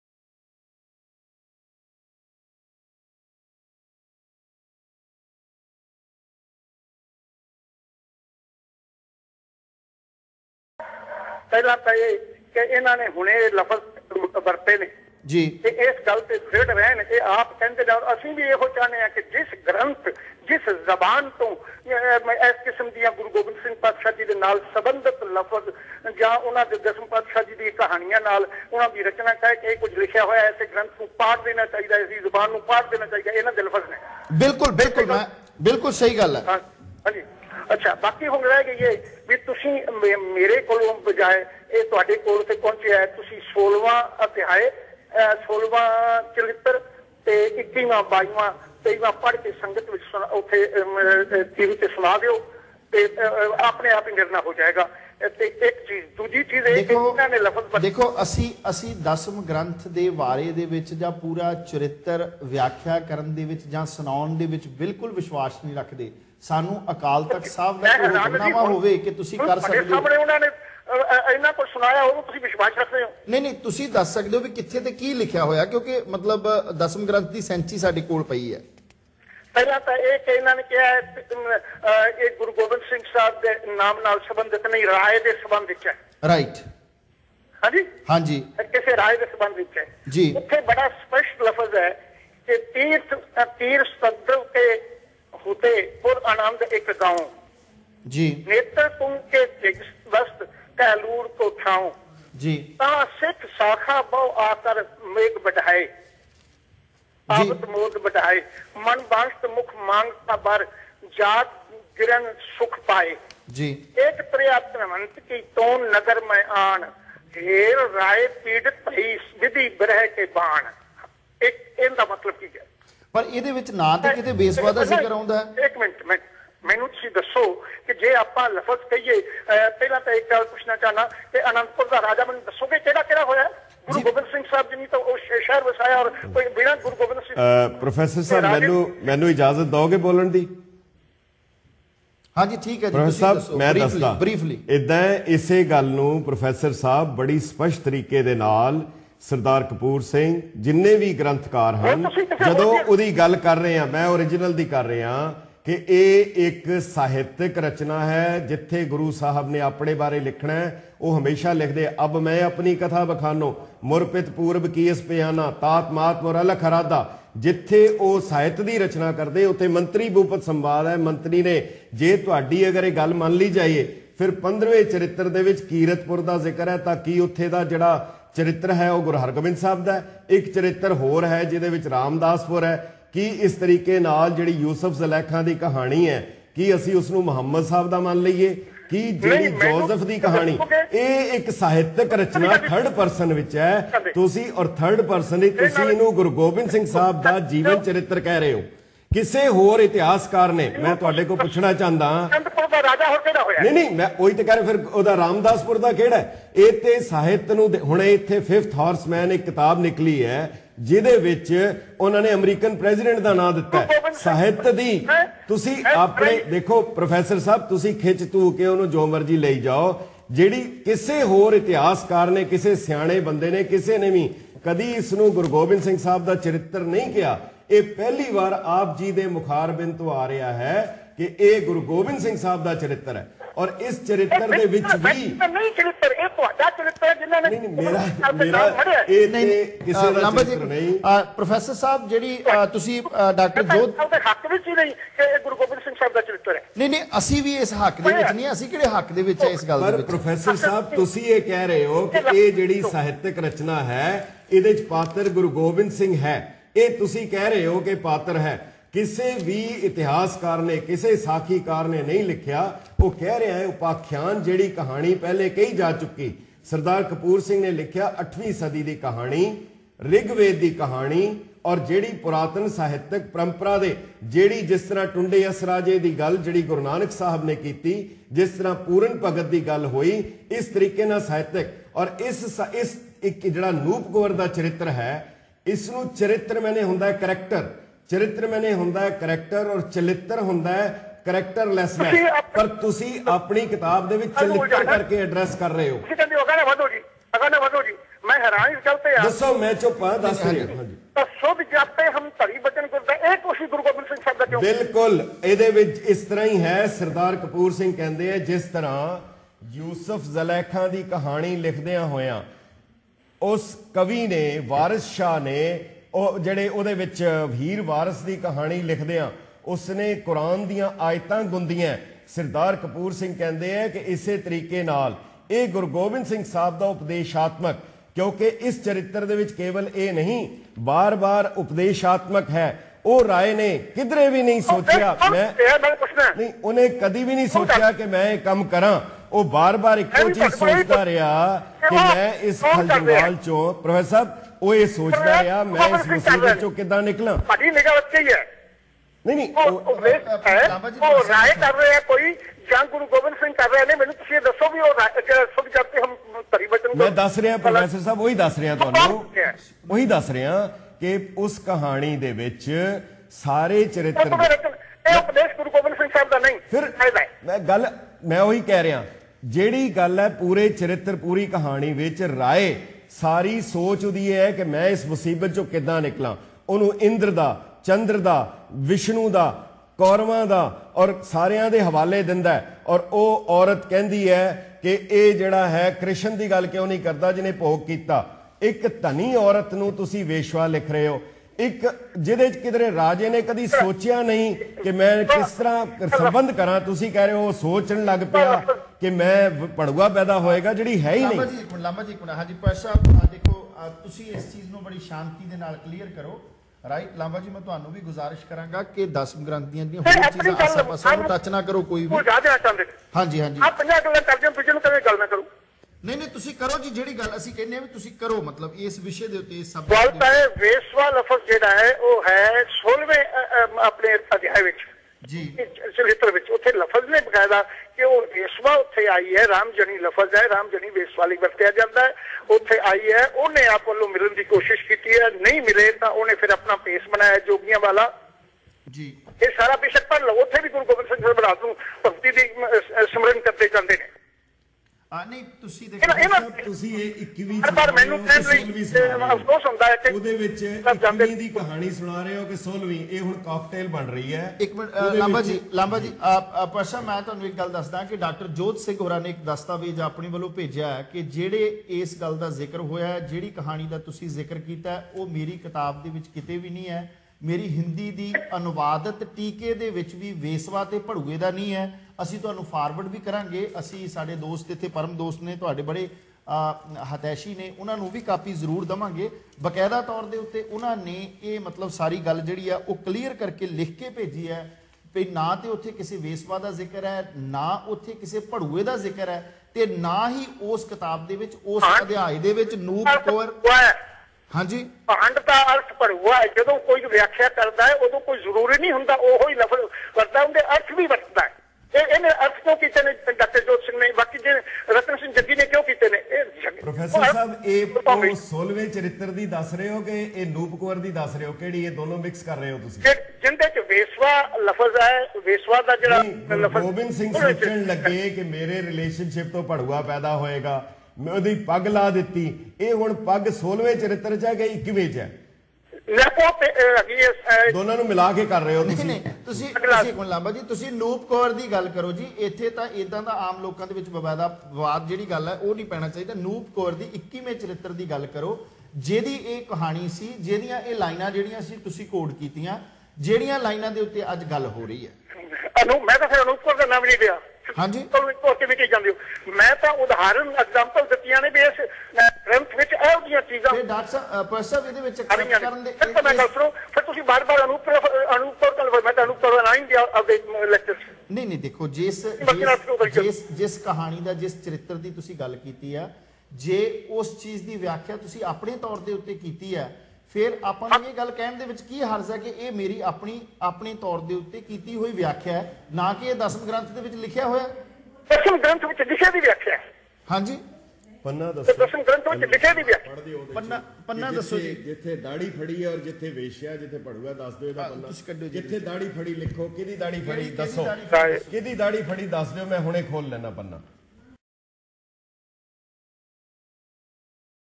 PART 3: Controversial Ragi Prof Darshan Singh talks about his blasphemous remarks on JUS Punjabi TV
Prof Darshan Singh Ragi was invited by JUS Punjabi TV Channel to clarify his position on the blasphemous remarks he made on the life of Sri Guru Gobind Singh Sahib.

While peforming at Rochester Gurdwara Controversial ragi Darshan Singh narrated a fictitious story denigrating the life and personality of Sri Guru Gobind Singh Sahib.

Darshan Singh said "Guru Sahib went to a 'Veshya' (prostitute) and the prostitute tried to lure him. Guru Sahib thought if he establishes relationship with her then the one who will be born out of that relationship will be a 'Bharua' (pimp) and if he doesnt establish a relationship then she will get him beat-up by the public by shouting 'thief thief'. Guru Sahib leaves his slippers and robe and starts running. He is caught by the public who starts beating him up. Someone gets hold of Guru Sahib's beard and someone removes his turban and starts hurling slippers on Guru Sahib's head"

He claimed that the story is from Charitropakhyan bani of Sri Dasam Granth. However no such narration with the name of Guru Sahib appears in Charitropakhyan.

Darshan Singh also used derogatory words such as 'Vesva' (prostitute) and 'Bharua' (pimp) which appear nowhere in the text.

The Sikh community strongly objected to his controversial remarks and Akal Takhat Sahib was requested to summon him.

It is not for the first time that the Ragi has surrounded himself in controversy. Earlier too he had casted aspersions on the Nitnem and Amrit Banees.